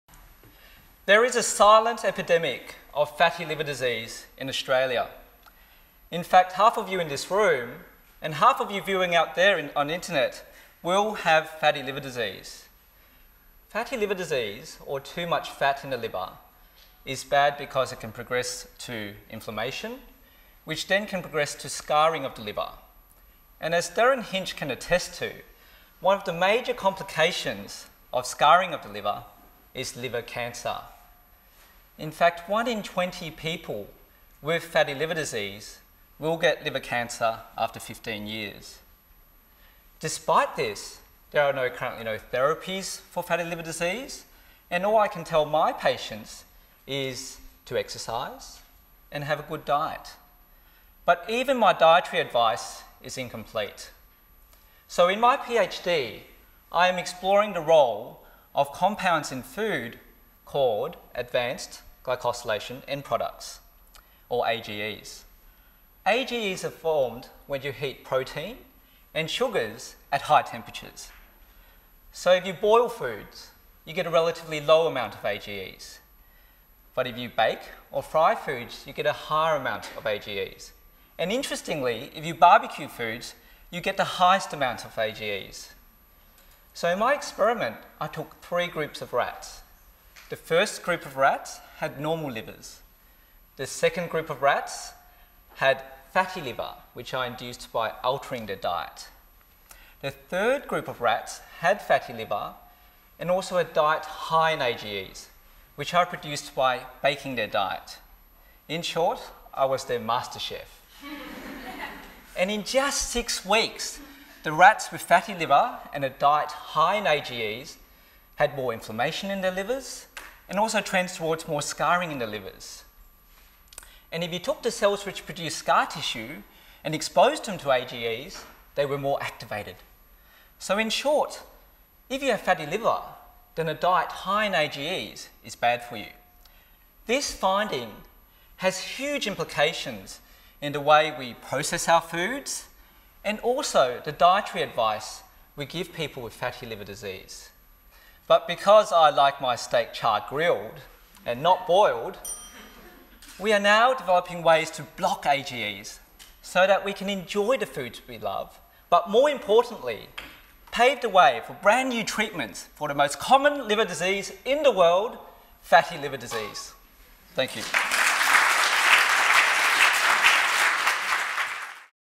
3 Minute Thesis - 3RD Prize - Chris Leung
Department of Medicine- 'Is our diet ageing us? The role of AGEs in fatty liver disease."